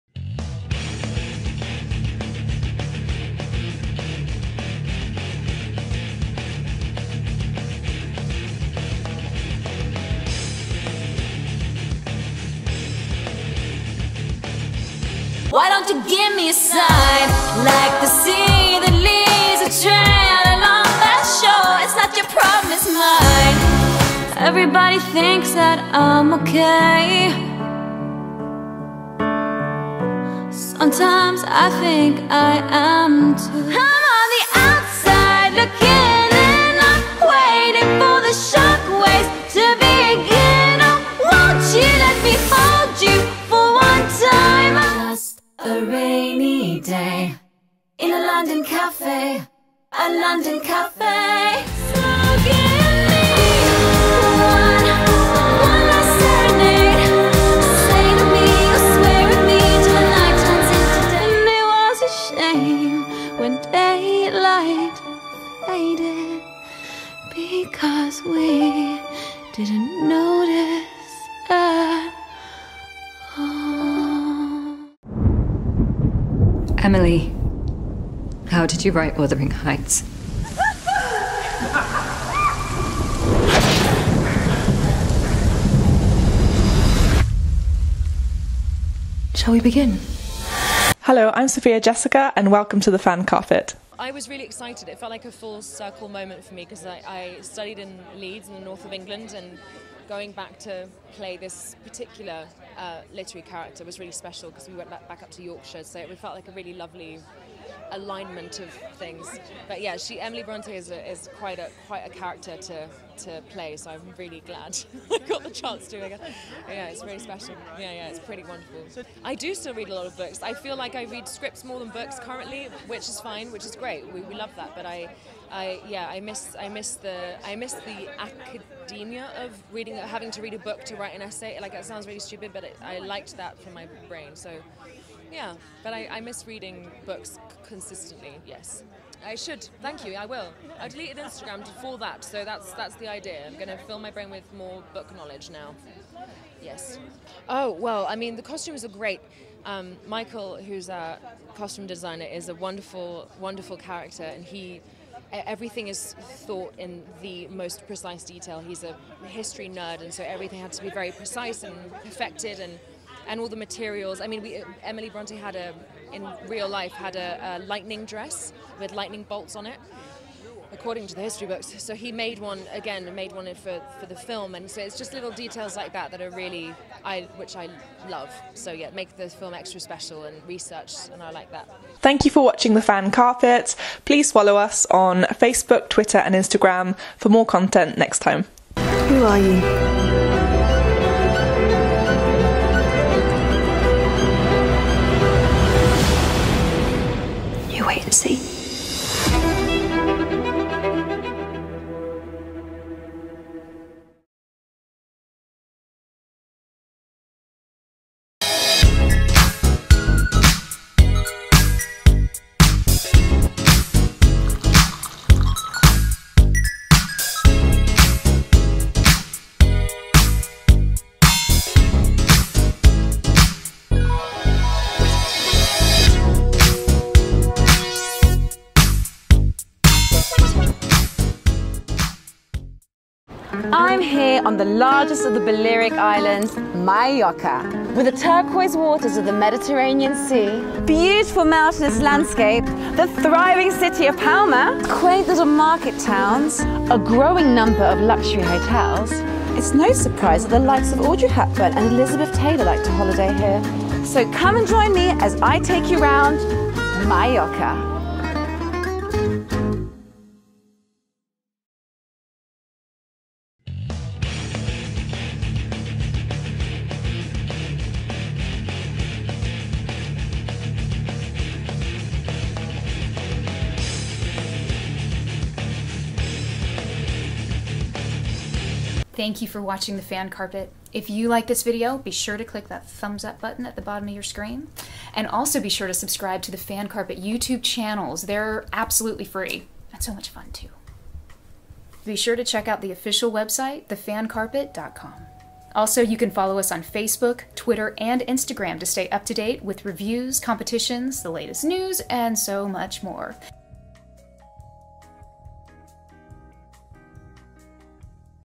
Toronto International Film Festival Premiere Clip: Emma Mackey | Emily (The Fan Carpet)
The Fan Carpet’s Sophia Jessica introduces our interview with Emma Mackey at the Premiere for EMILY at the Toronto International Film Festival.

“EMILY” tells the imagined life of one of the world’s most famous authors, Emily Brontë. The film stars Emma Mackey (“Sex Education”, “Death on the Nile”) as Emily, a rebel and misfit, as she finds her voice and writes the literary classic Wuthering Heights. “EMILY” explores the relationships that inspired her – her raw, passionate sisterhood with Charlotte (Alexandra Dowling – “The Musketeers”) and Anne (Amelia Gething – “The Spanish Princess”); her first aching, forbidden love for Weightman (Oliver Jackson-Cohen – “The Lost Daughter”, “The Haunting of Bly Manor”) and her care for her maverick brother (Fionn Whitehead – “The Duke”, “Dunkirk”) whom she idolises.

Frances O’Connor makes her directorial debut with “EMILY,” from her own original screenplay.

The film also stars Adrian Dunbar (“Line of Duty”) and Gemma Jones (“Bridget Jones’s Baby”, “Gentleman Jack”).

EMILY will be released in cinemas on 14th October by Warner Bros. Pictures.